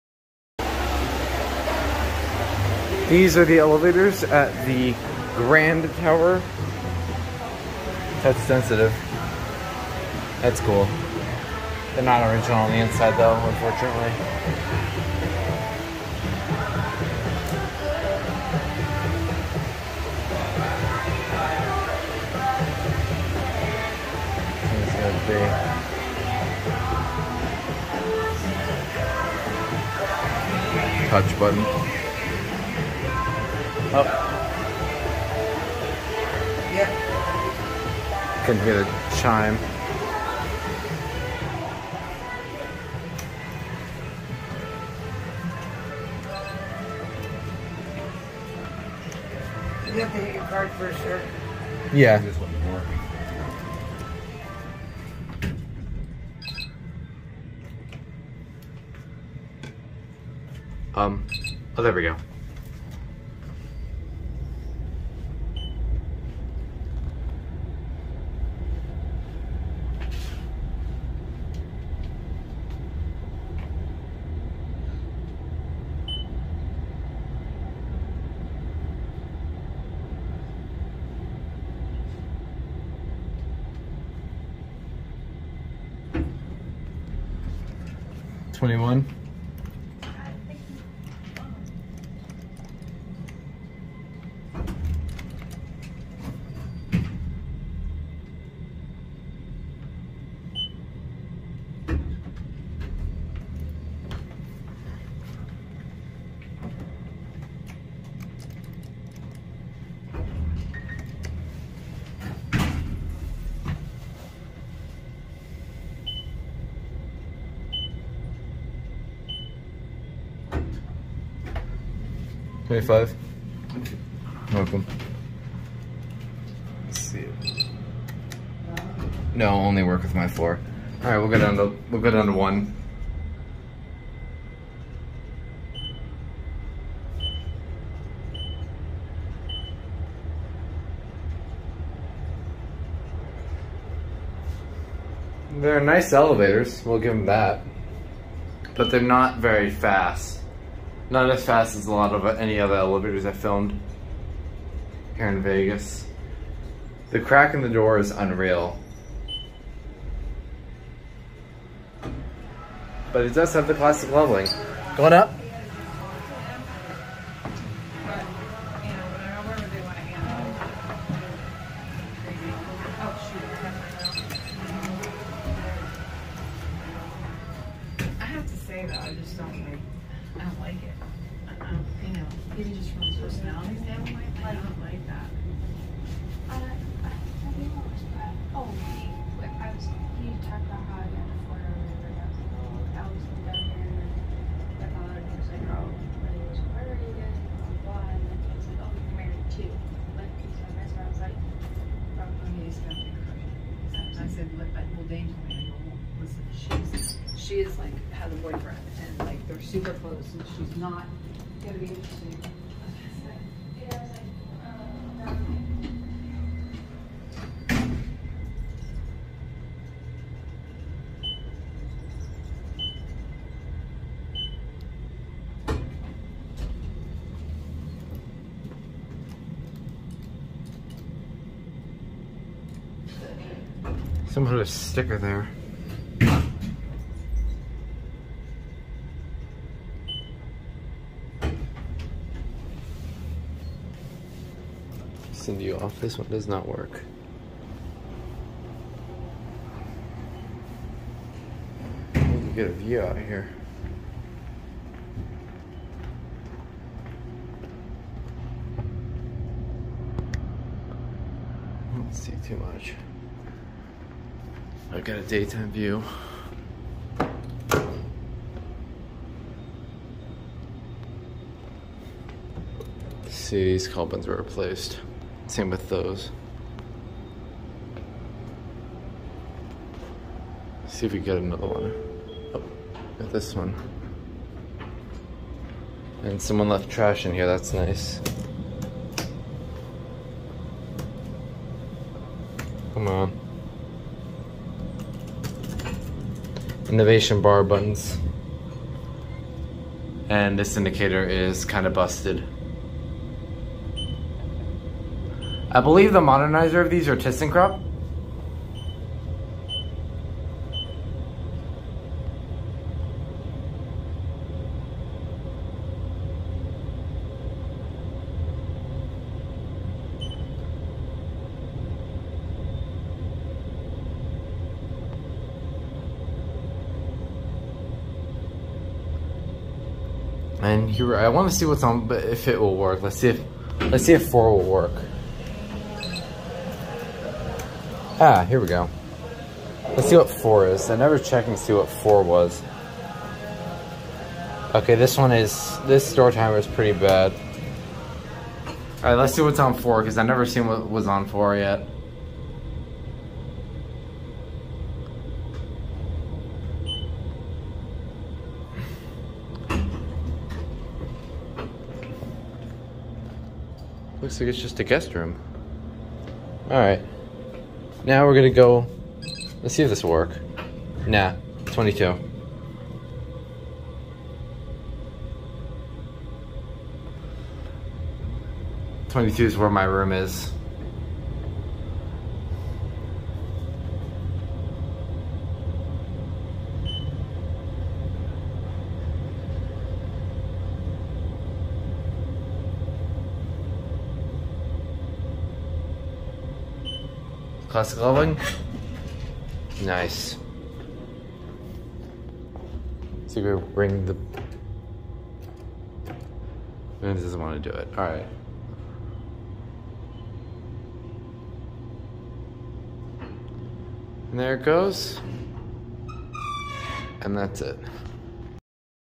Otis Traction Grand Tower Elevators @ MGM Grand, Las Vegas NV
These got smart carded but I somehow made it to the top. Even if not I was a guest at this hotel during my trip.

Tech Specs:
Brand: Thyssenkrupp?
Originally: Otis Series 3
Maintenance: ??
Model/Fixture: Innovation Prestige
Type: Traction
Floors Served: 1, 4, 21-29
DT Rate: 1/10 Slower than it should be